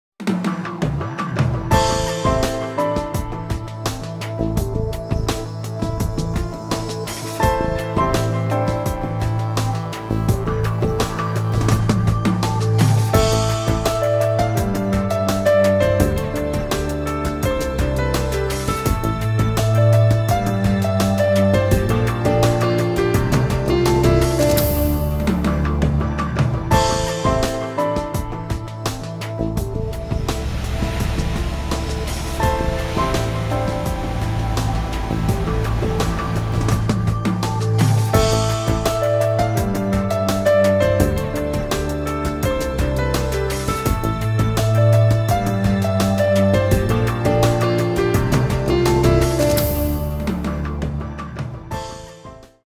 Kaugummientfernung mit Aquila Hochdruckreiniger
Probereinigung von verschmutzten Betonwerksteinen in Hannovers Innenstadt. Das Ergebnis ist überzeugend.